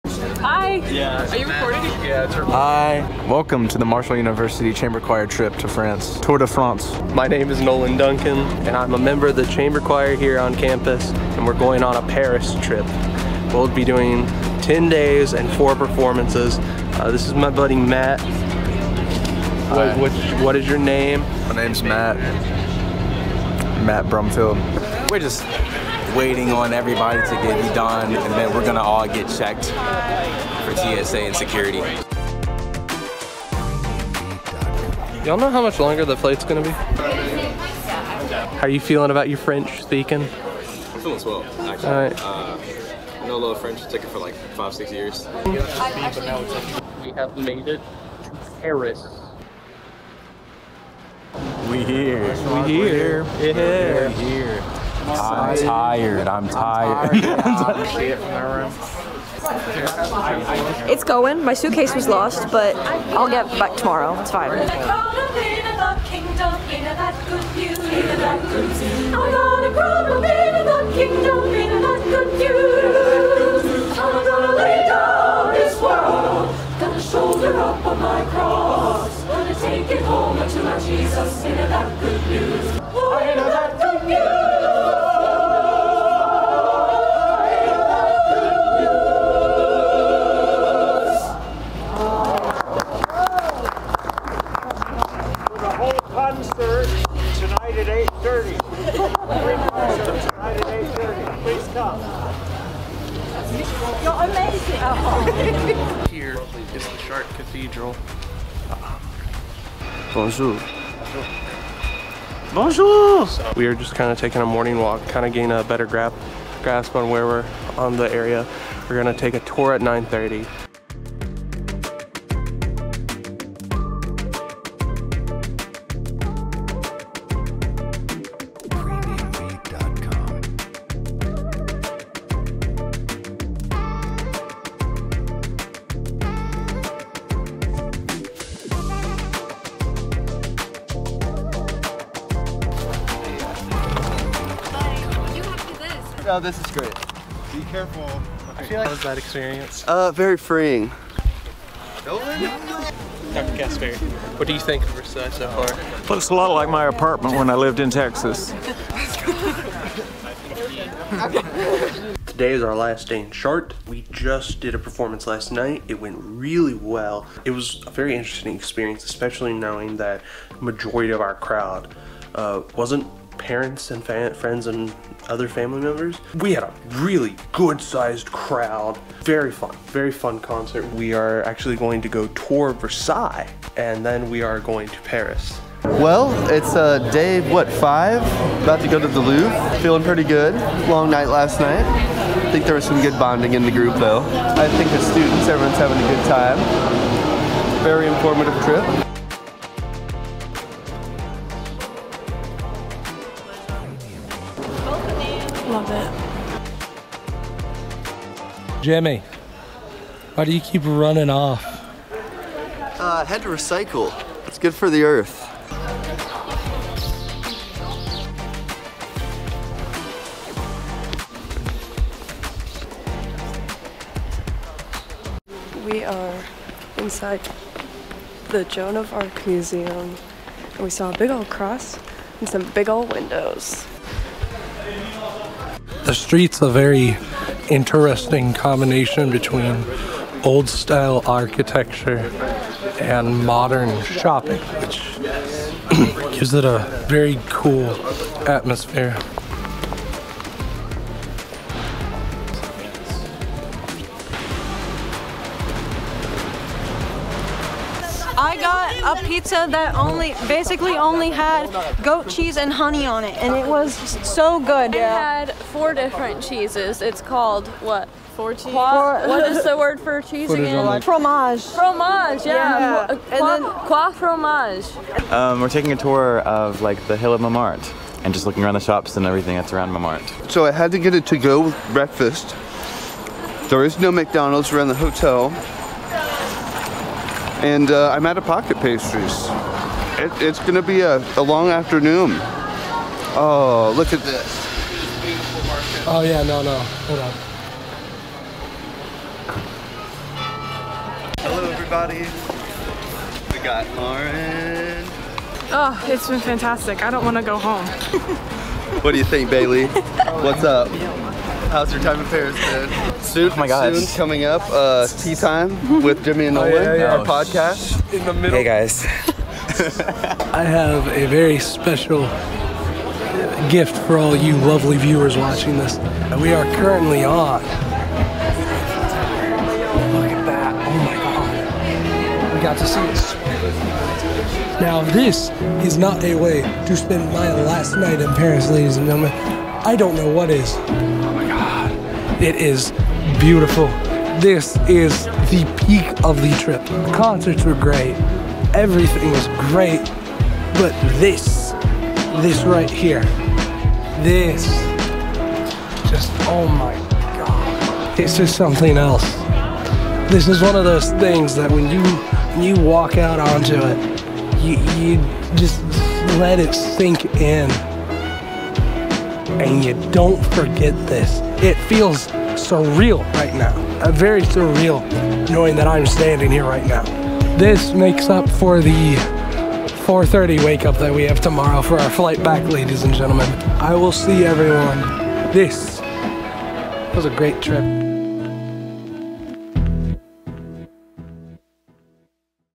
Choir France Trip Vlog | Hear From the Herd
The Marshall University choir traveled to France and performed at Cathédrale Notre-Dame de Chartres, built in the 13th Century.

In addition to concerts, the choir toured the Louvre Museum, visited the famous Latin Quarter, took a boat ride on the river Seine, and made a tour of the Palace of Versailles.

Following along as they share their trip in the most recent edition of Hear From the Herd

Find out more about available scholarships, majors, campus visits and applying